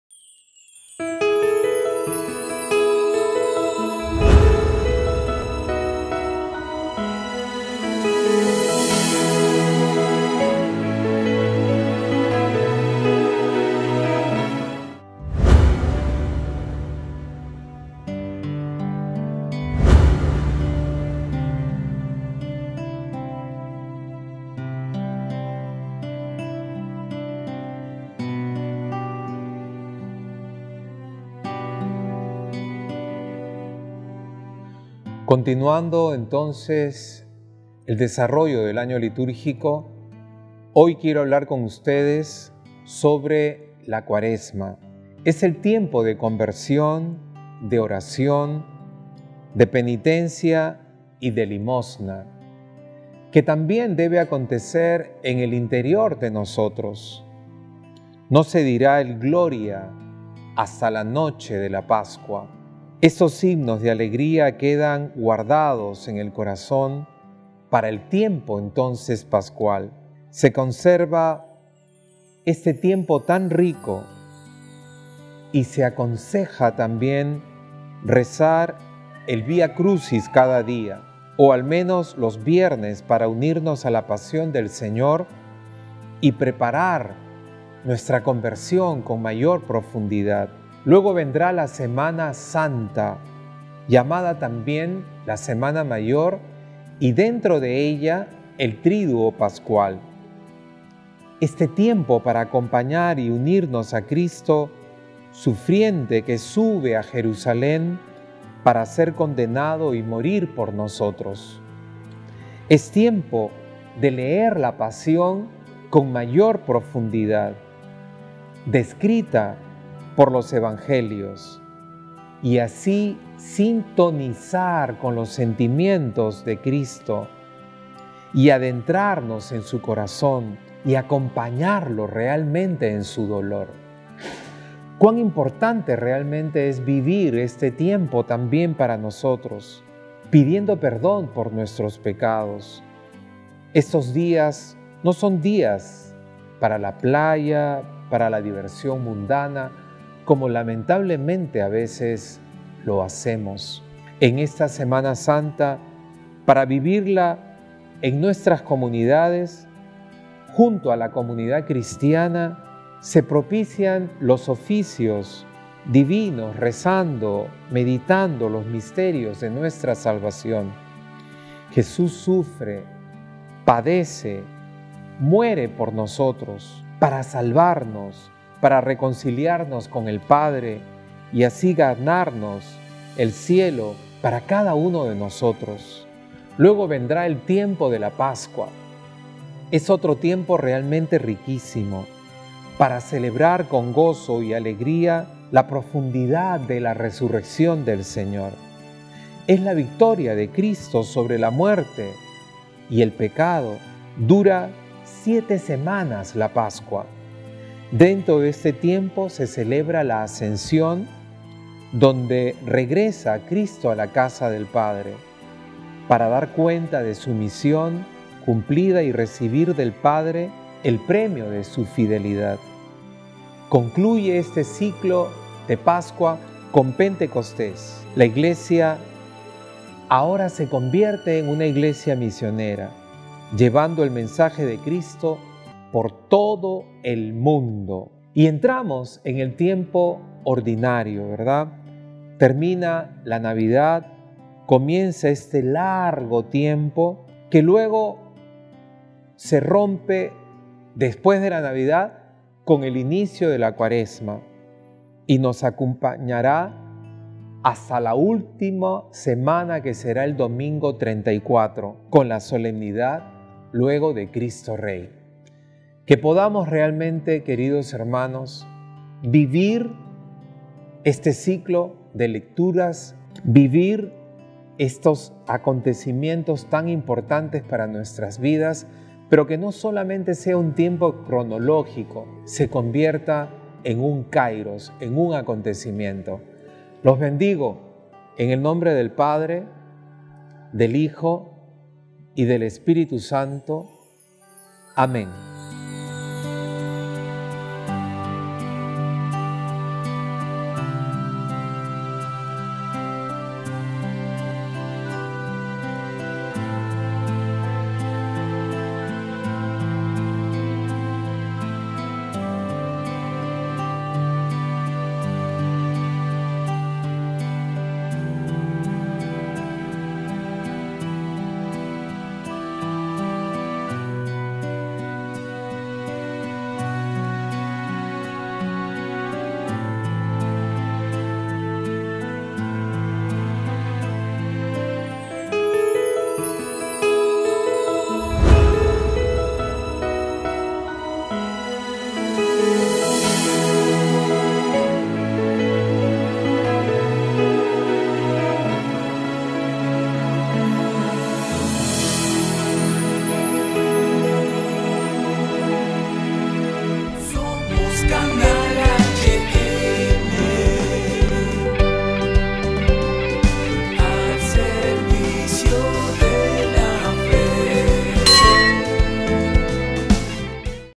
Canal HN, Palabra de Fe: Año Litúrgico
Canal HN
Palabra de Fe: Año Litúrgico
Compartamos juntos, hoy jueves 19 de noviembre, la Palabra de Fe: Año Litúrgico, con el tema "Cuaresma semana mayor, Tiempo Pascual y Tiempo Ordinario" dadas por el Mons. Guillermo Elias Millares, Obispo Auxiliar de Lima